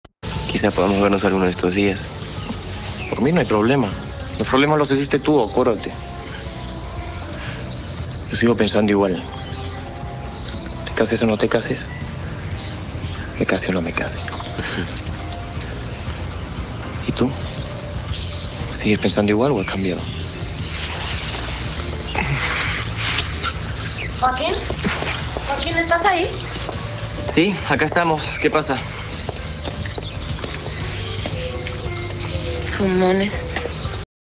No se lo digas a nadie
que tal chape de christian a magil, parece que lo disfrutaron caracho!